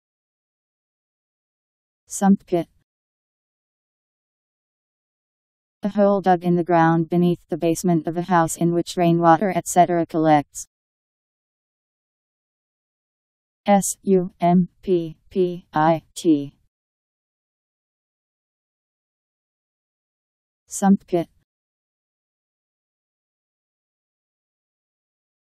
Sump pit Meaning
Video shows what sump pit means. a hole dug in the ground beneath the basement of a house in which rainwater etc collects.  Sump pit Meaning. How to pronounce, definition audio dictionary. How to say sump pit. Powered by MaryTTS, Wiktionary